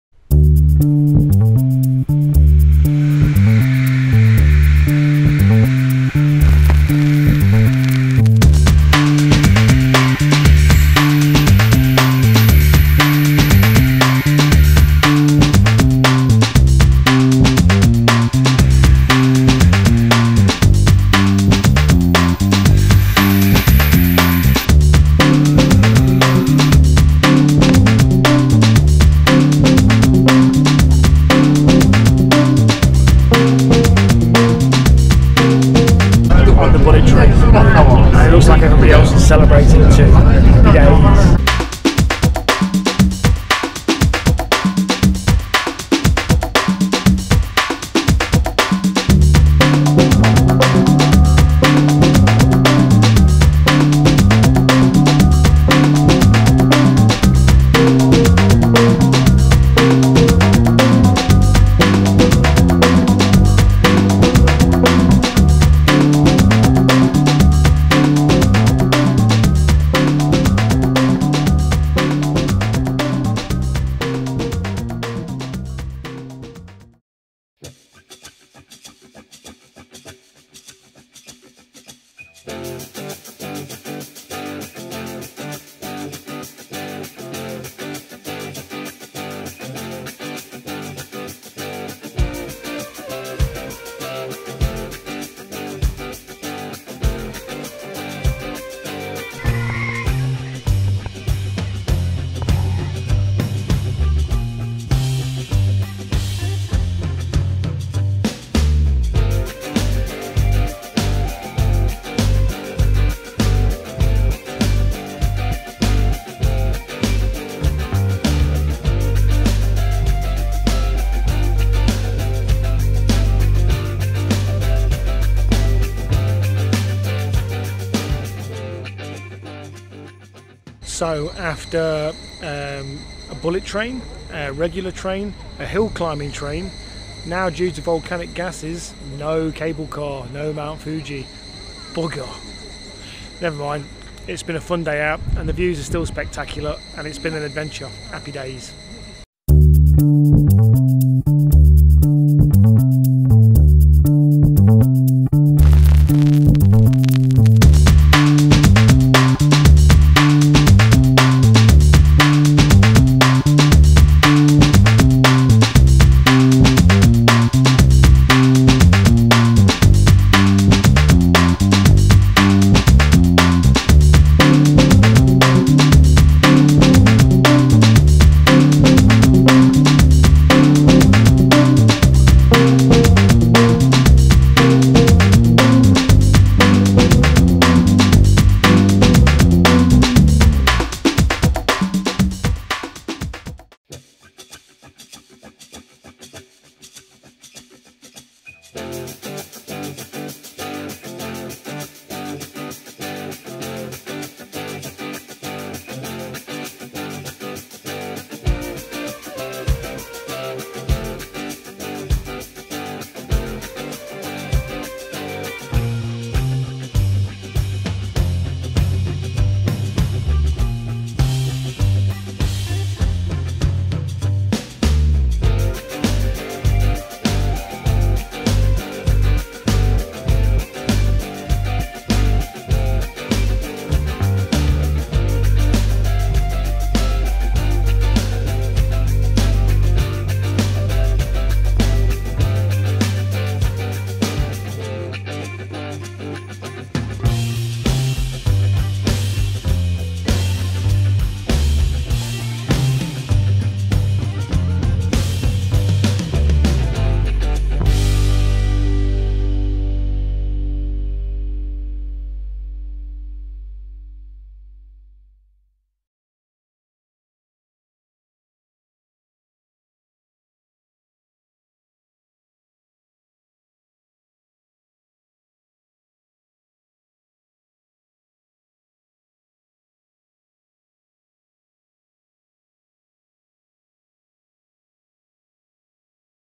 5 nights in Tokyo Part2, Bullet Train, Odawara Castle & Hakone-VLOG 8
bullet trains and more trains, Odawara Castle and Hakone